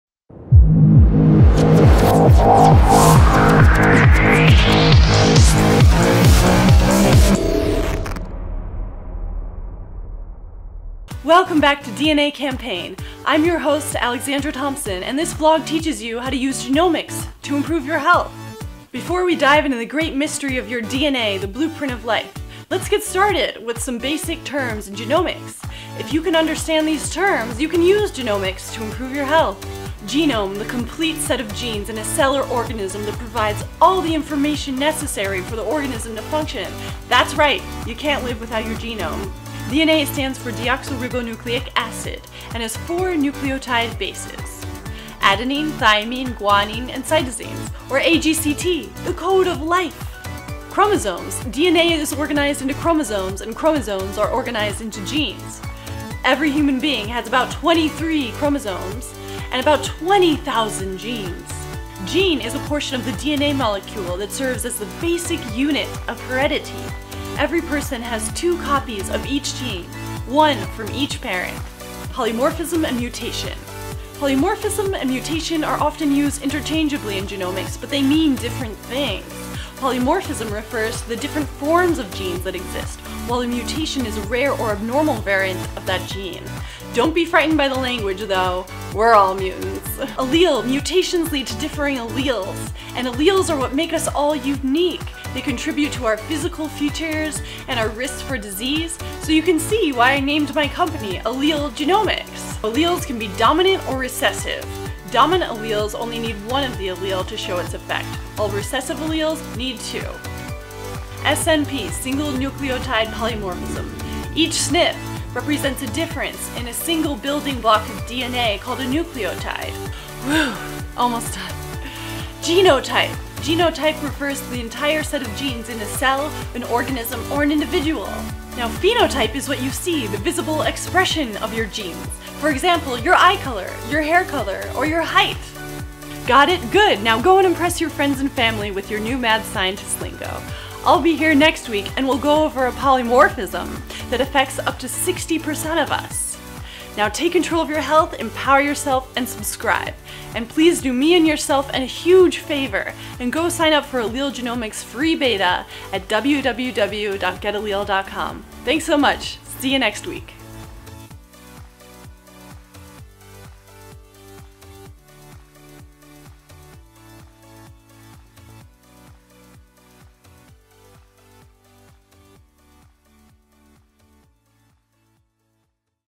Learn Genomics in 2 Minutes ✔️
This show is about empowering YOU to improve your health with genomics. In this episode, we learn the most essential terms and definitions in genomics. Once you understand these terms, you'll be able to use them to improve your health...and up your mad scientist cred!